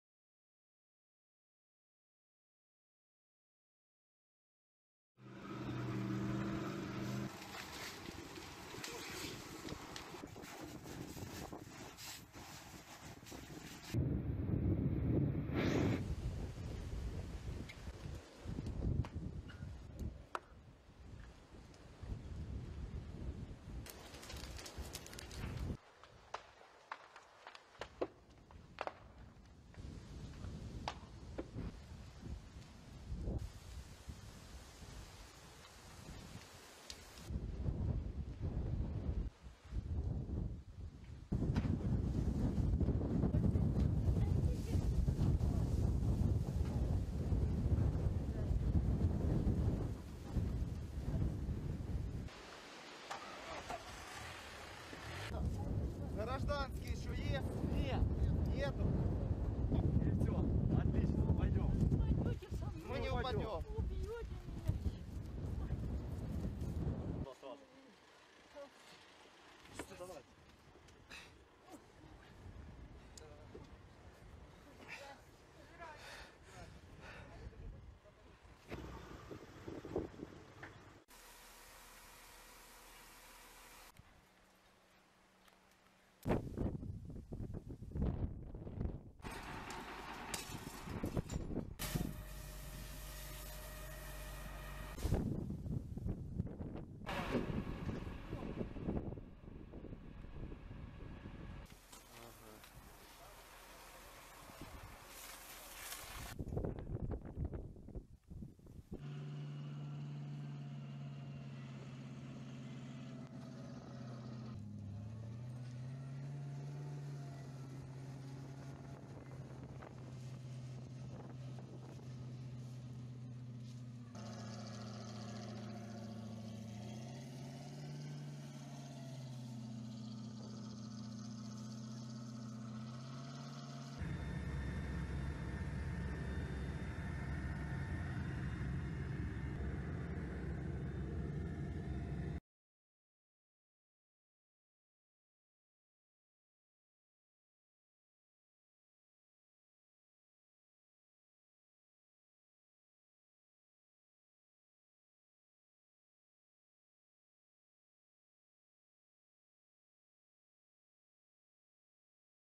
Ukraine Wildfires 2020: Forest fires in eastern Ukraine kill at least 9 people
At least nine people have died and ten hospitalized after a series of forest fires swept through part of the eastern Ukrainian region of Luhansk, Ukraine's emergency service said on Thursday..

The fires threatened 22 settlements and engulfed an area of about 18,000 hectares (70 sq miles), the service said in a statement, adding that 120 people had been evacuated.

Local authorities said there were a total of 146 fires, 82 of which have been extinguished. October 1st, 2020.